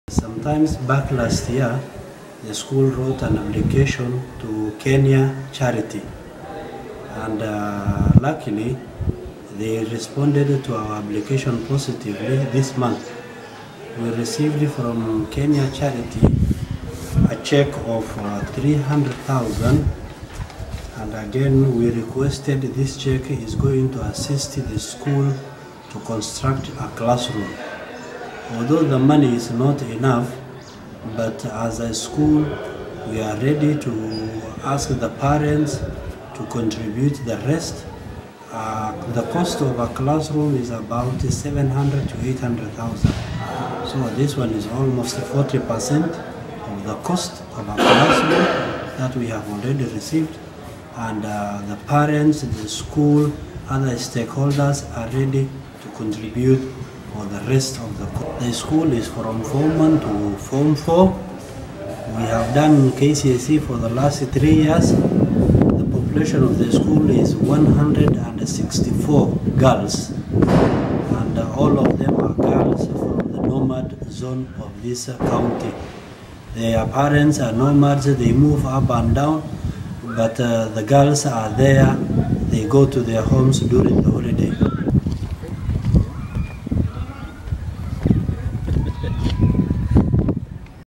A girls school in Mandera benefited from a lottery donation of Sh300,000
A girls'  school has benefited from a lottery donation of Sh300,000.
Mr. Abdinoor Billow Wadese, Ashabito Girls Secondary School Principal said the donation shall be used in constructing a classroom for the girls.
The donation is to promote girl child education in Mandera County.